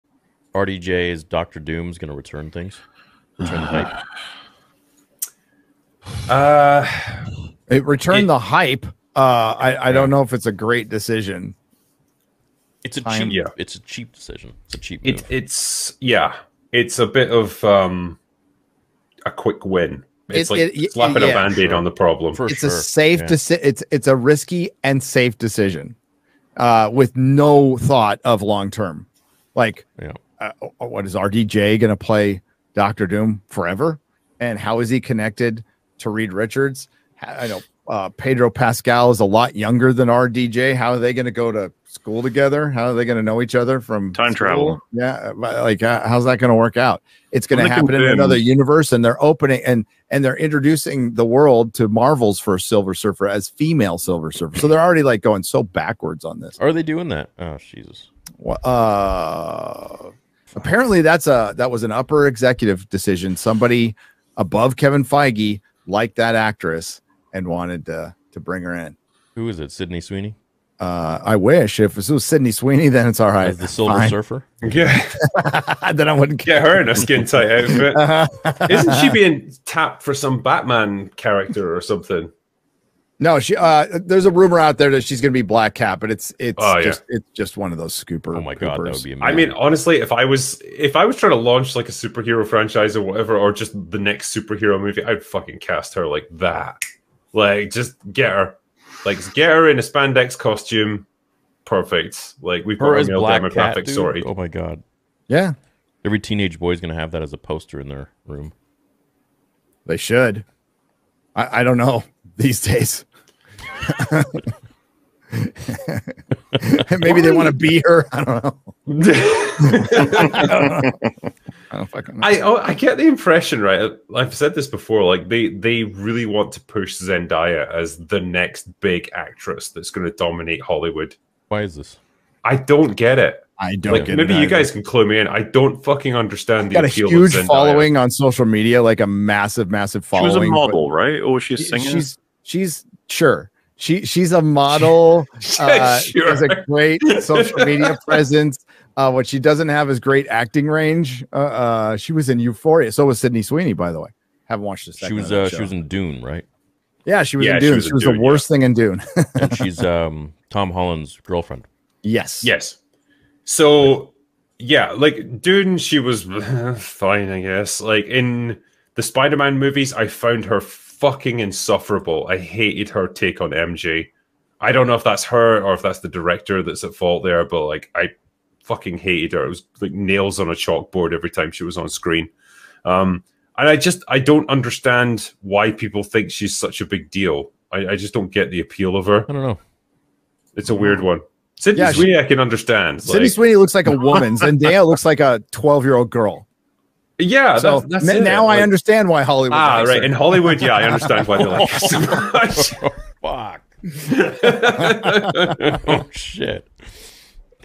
Critical Drinker Can't Comprehend Hollywood Pushing Of Zendaya Over Sydney Sweeney
On Open Bar Drinker, Mauler, Star Wars Theory And Nerdrotic talk about why Hollywood seems to prefer Zendaya to Sydney Sweeney.